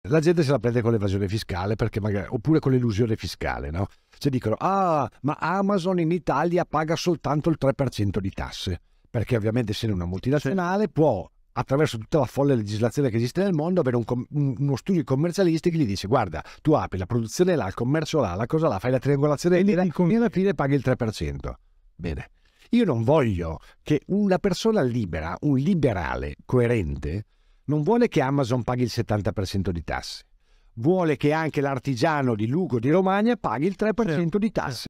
Amazon paga poche tasse in Italia?
Chissà come mai c'è sempre chi vuole che Amazon paghi più tasse e nessuo che chieda che l'artigiano paghi le stesse tasse di Amazon.

✅ Sostieni lo Show: offrimi un caffè!